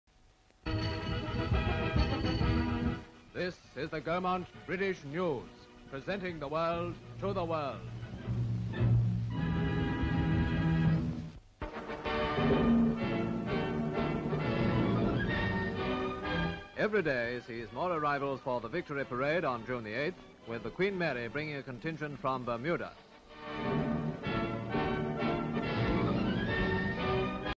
Troops from Bermuda land in Britain for Victory Day parade (1946)
Bermudan troops arrive on ocean liner SS Queen Mary for Victory Day parade in Britain

SLATE INFORMATION: More Victory Day Arrivals in Britain

ENGLAND: Southampton:
EXT

BERMUDA Troops from Bermuda arrive at Southampton on "Queen Mary"

"QUEEN MARY" Ship Arrives at Southampton With troops from Bermuda & Lord Halifax & Mr MacKenzie King on Board

VICTORY. (GREAT WAR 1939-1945) Victory parade arrivals from Fiji, Congo & Bermuda

Celebrations - Victory; Military - Active

Background: Bermudan troops arrive on ocean liner SS Queen Mary for Victory Day parade in Britain